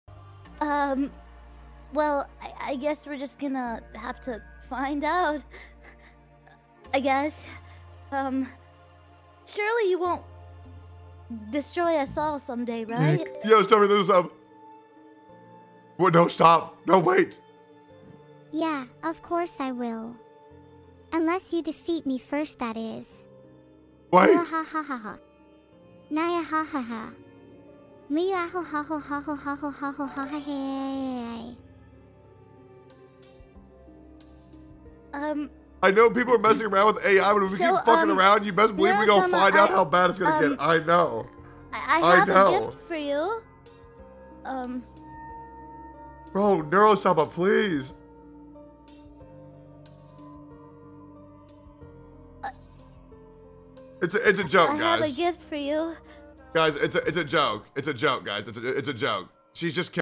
A.I is getting TooREAL 😱 Tectone watch Neuro sama & Camila Collab.
- Clipped by MisterAngleo0